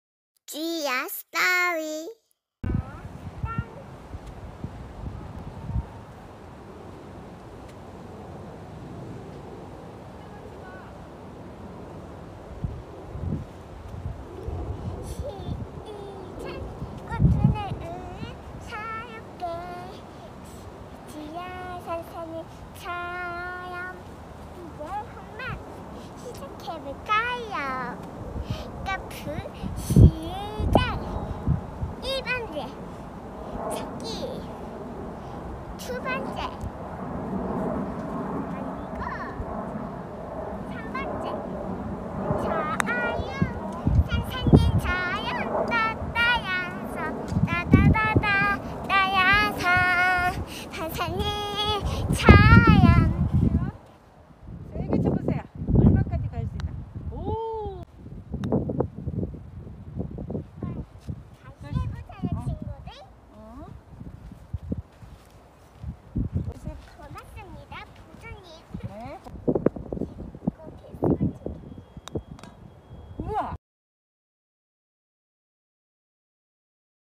골프 는이렇게!!
퍼터장에서 지아가 노래 부르며 즐겁게 골프 연습합니다